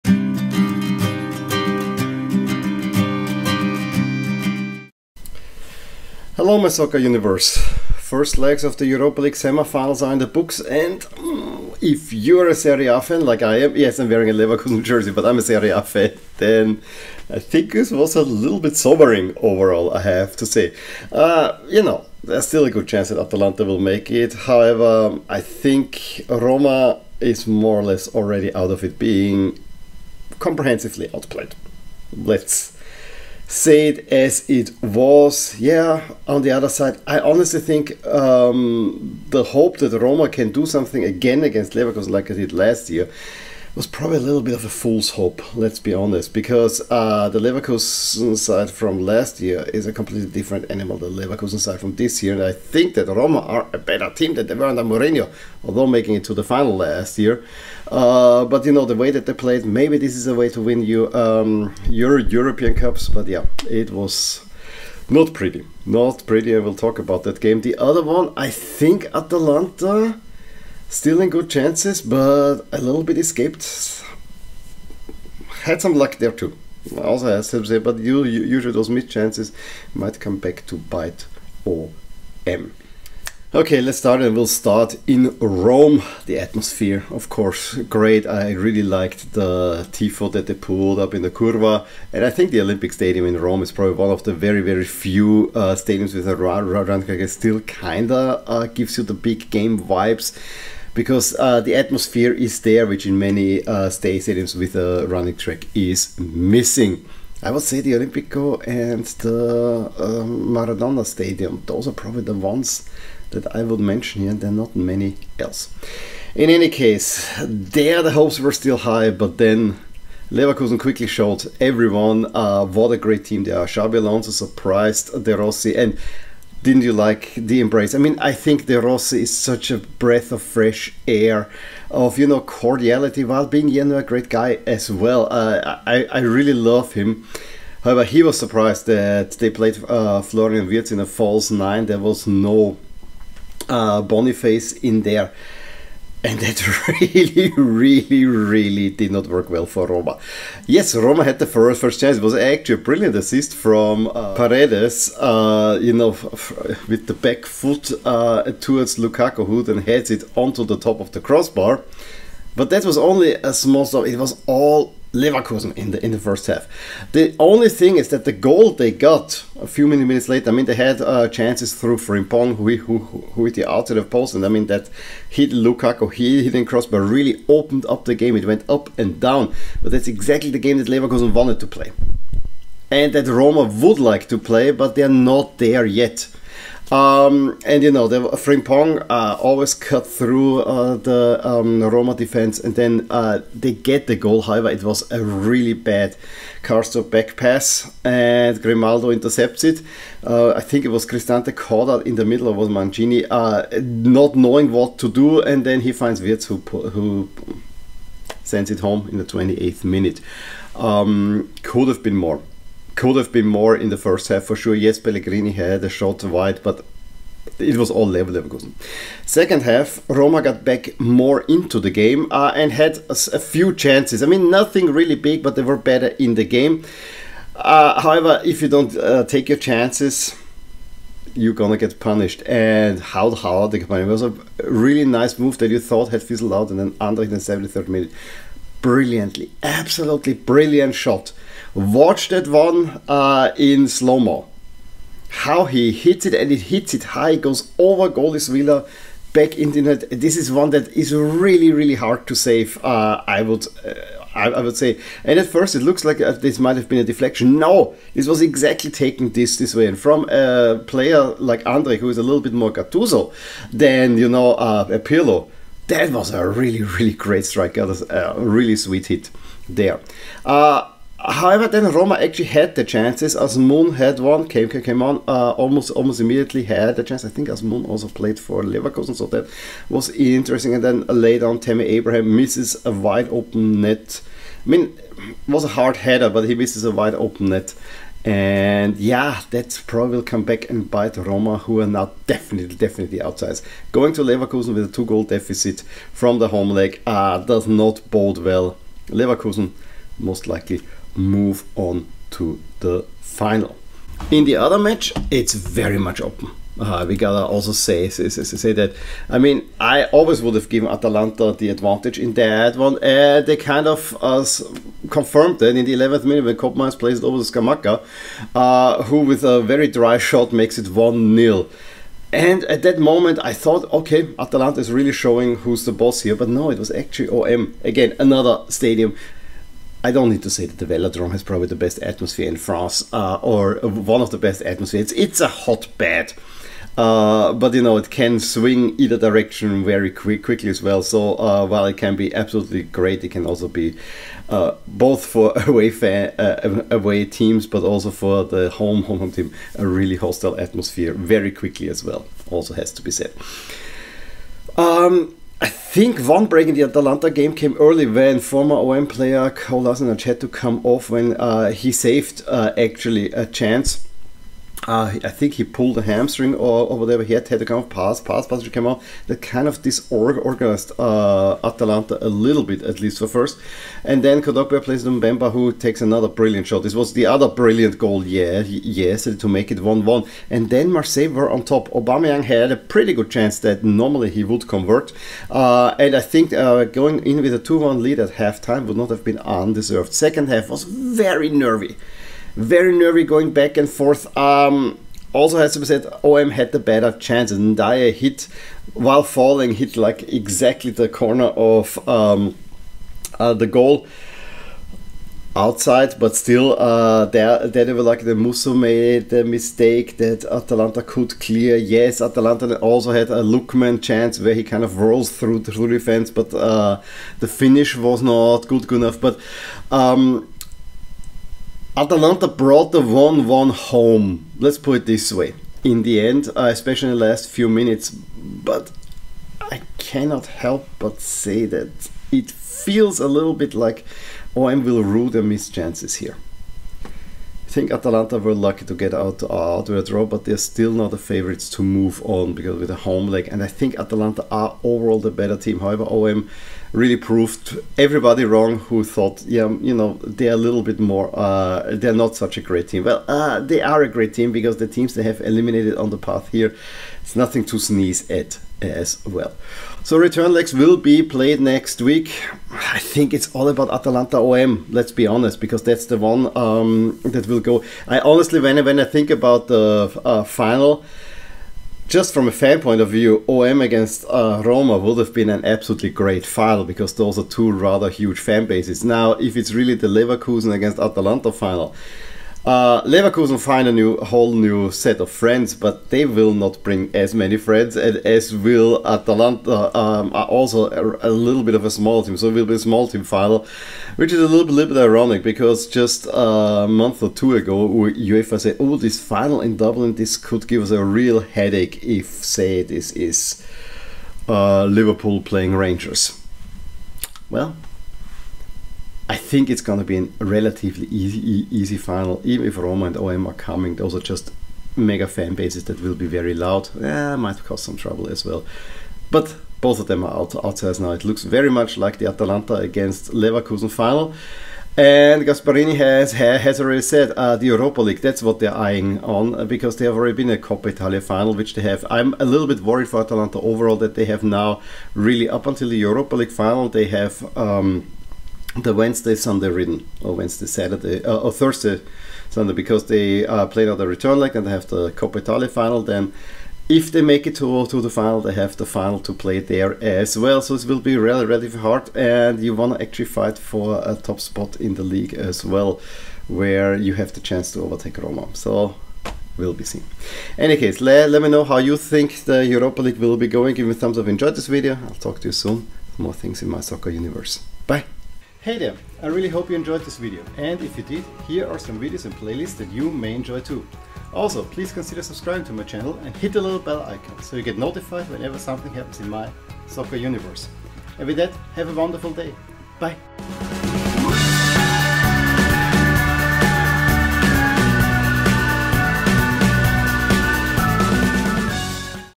A Sobering Evening for Serie A
If you were a fan of Serie A, then this evening was a sobering affair: Romaa never really stood a chance against an all conquering Leverkusen side, while Atalanta in the end were a but lucky to get out with a draw on their visit at the Velodrome.

00:00 Opening observations
01:23 Leverkusen overwhelm Roma
05:04 OM cannot find a winner over Atalanta
07:39 Concluding remarks